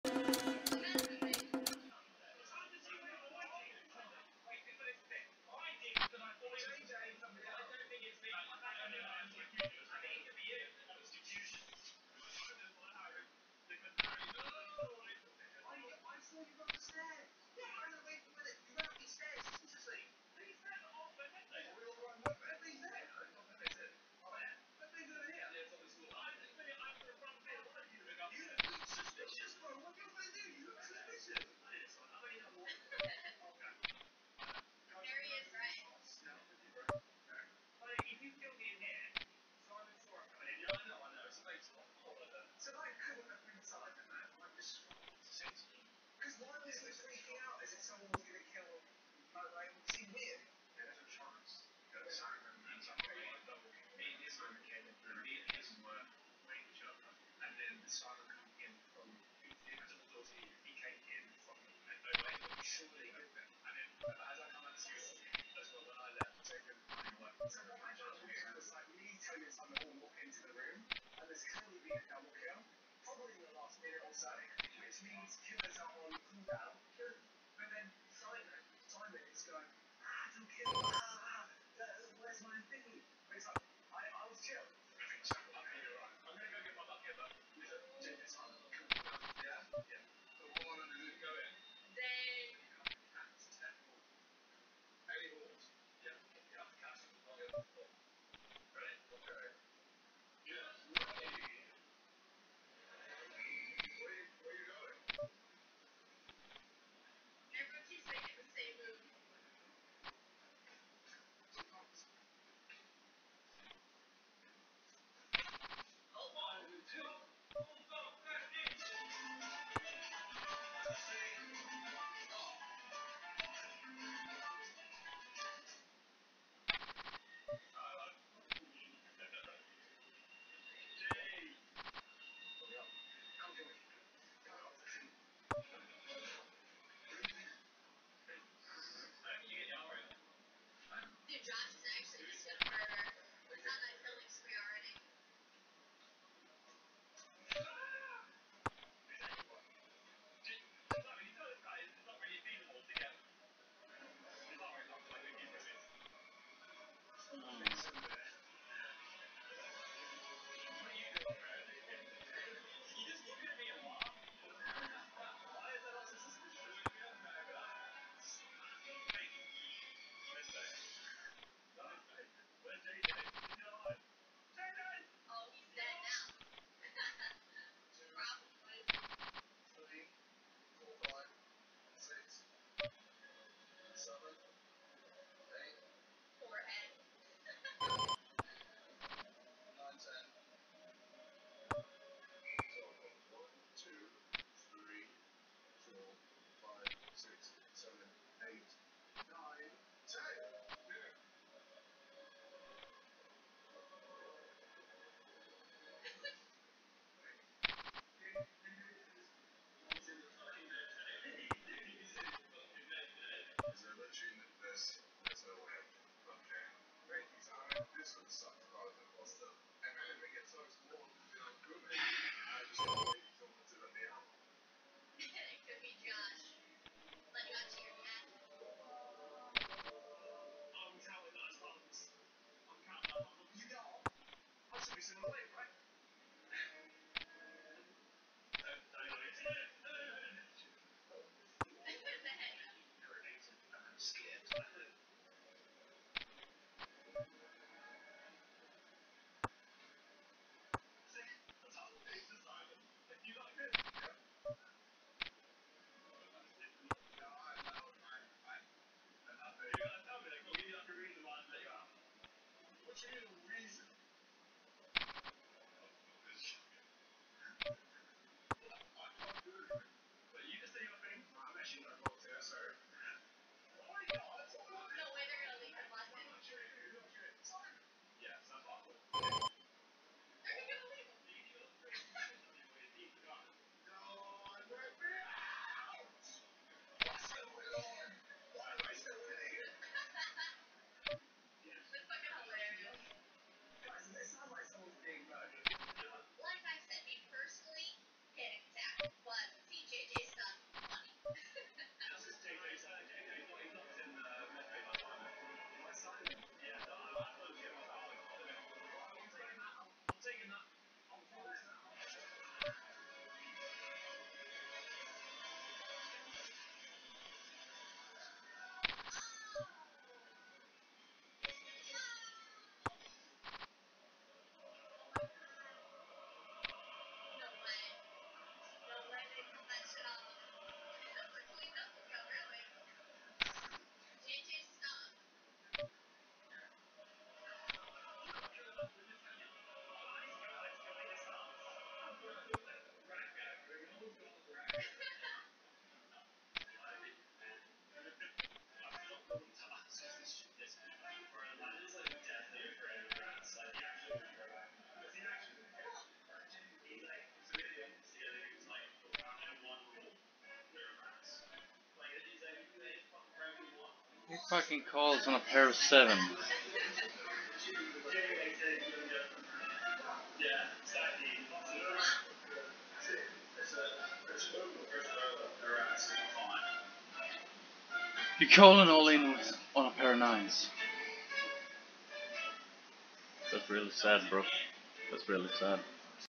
INSANE CALL FROM HIM, HE READ MY STONE COLD BLUFF.. Poker Rags to Riches Episode 112
that is why I am doing well because people call my all in shoves with middle pair on the river.

I just got to actually have it.